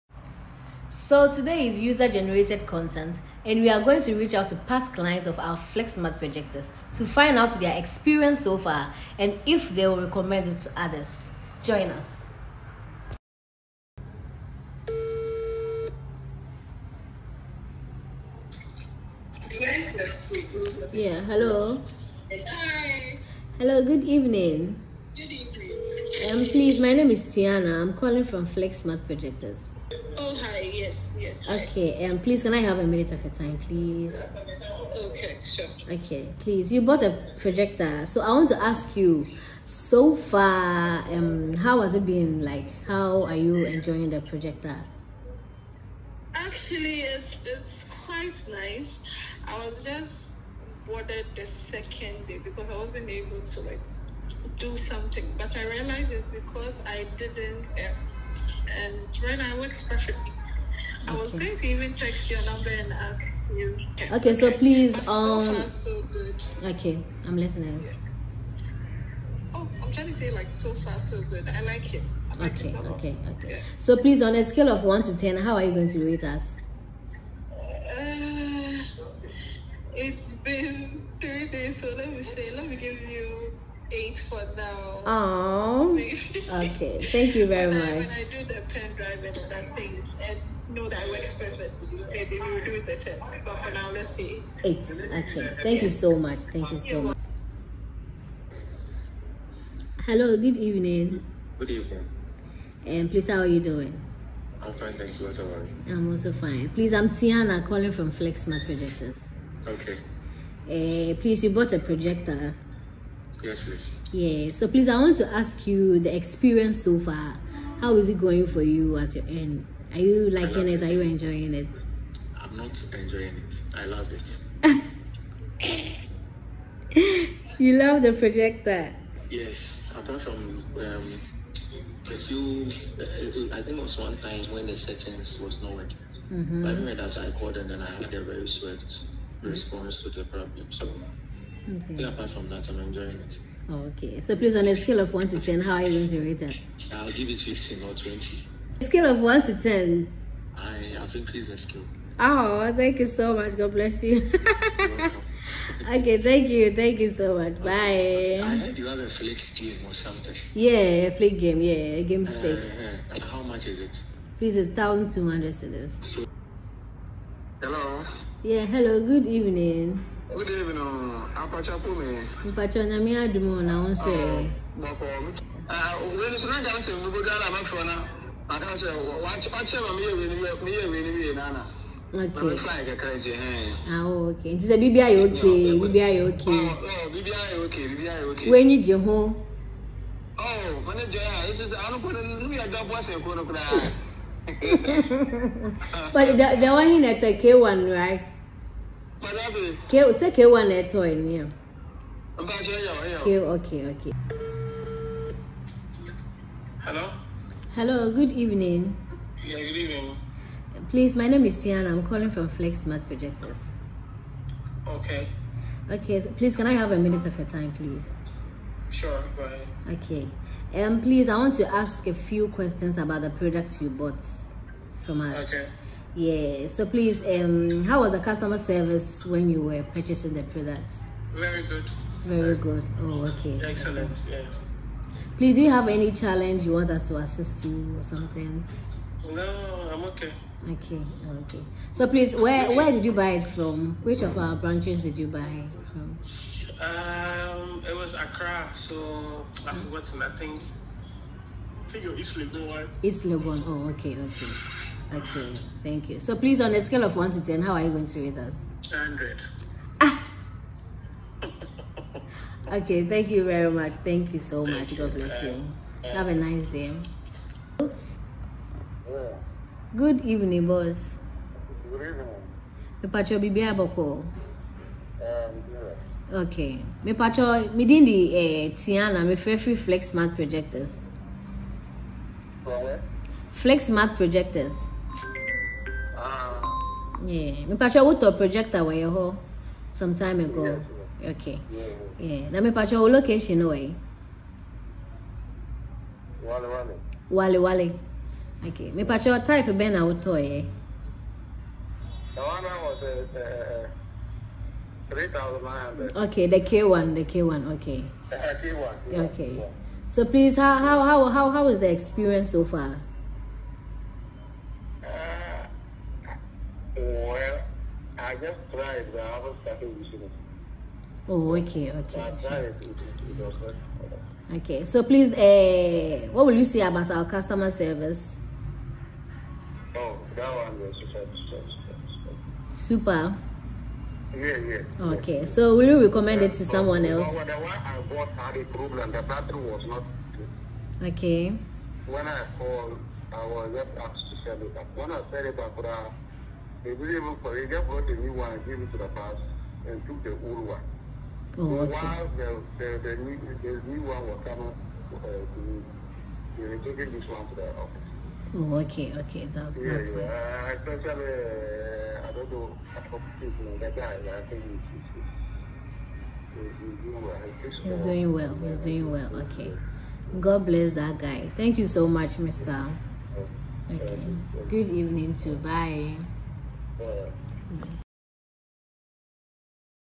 Flick Smart Projector Reviews By Users
Here are some random reviews of users who bought flick mini projector .